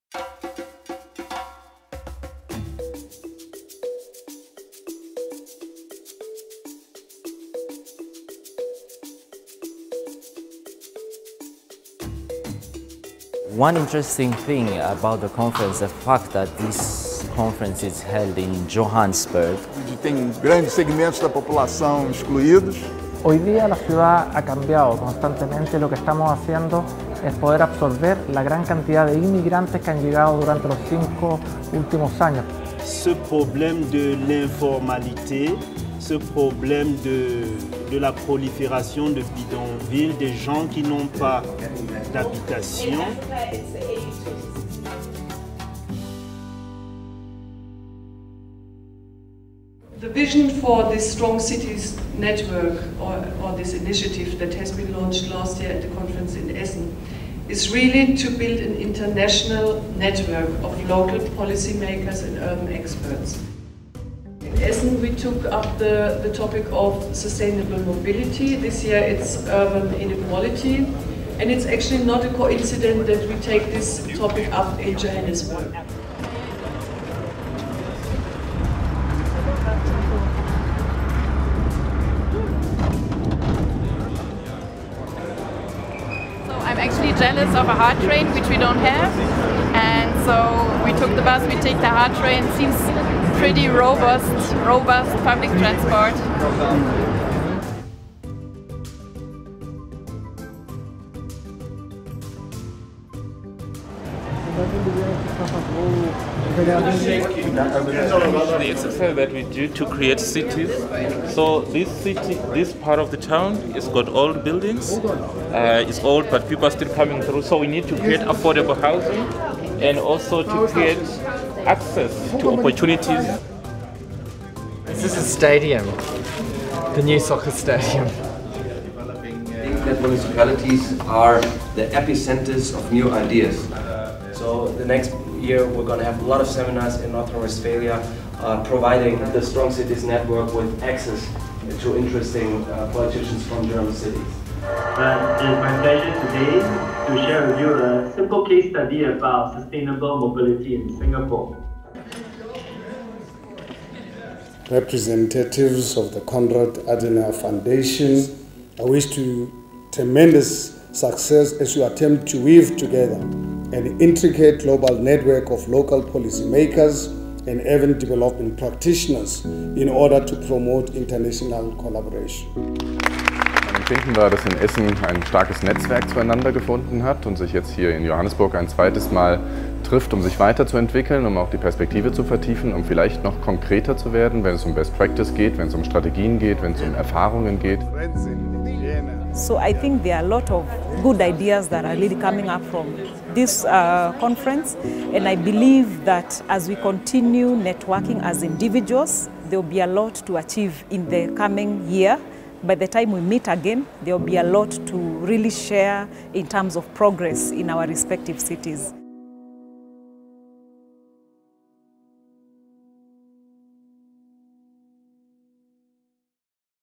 Strong Cities 2030 - Innovative approaches to overcome urban inequality
Cities around the world are faced with the challenge of growing spatial, social and economic inequality. The Strong Cities 2030 conference that took place from 13-15 October 2019 in Johannesburg, South Africa provided a platform for young local politicians and urban experts from around the globe to exchange experiences, share best practices and develop new creative ideas on how to overcome urban inequality.